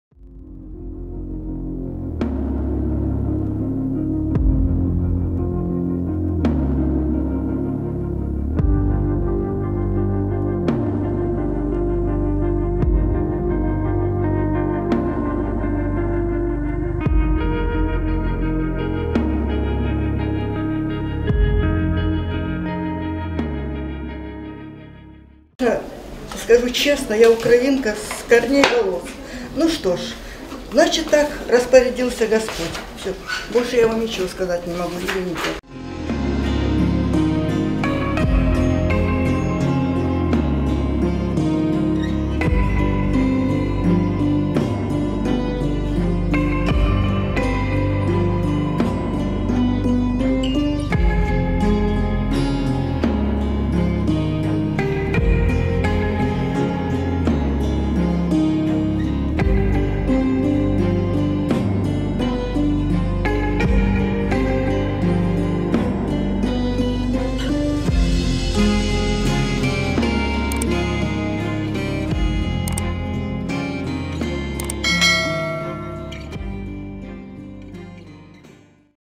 Ribuan Warga Ukraina Telah Terima Paspor Rusia
Beberapa penduduk di Berdyansk Ukriania telah diberikan paspor Rusia.
Menurut layanan migrasi departemen kepolisian Berdyansk sekitar 2.500 warga Ukraina mendapat paspor Rusia sejak Juni. Sementara 5.000 lainnya masih menunggu untuk dinaturalisasi.

Seorang pensiunan bernama  Nadezhda Vychea mengatakan bahwa ia adalah orang Ukraina dari atas hingga ujung kaki nya. Namun karena kehendak Tuhan ia mengambil kewarganegaraan Rusia.
Penulis Naskah: Novyana Nurmita Dewi
Video Editor: Novyana Nurmita Dewi
Produser: Firzha Ananda Putri